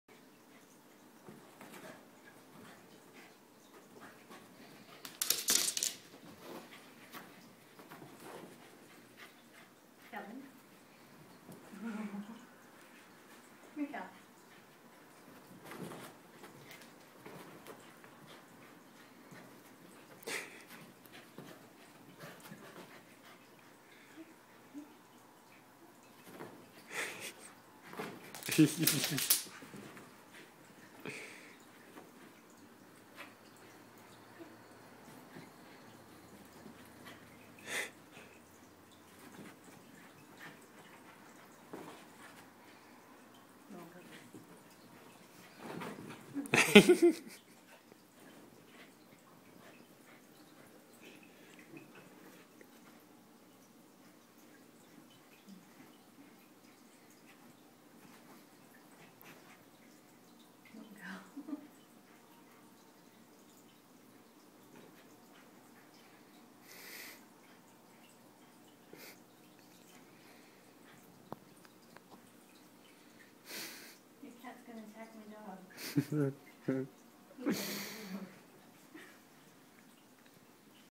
Corgi and Siamese chase laser
Watch this Corgi chase the laser while the Siamese chases the Corgi...too funny!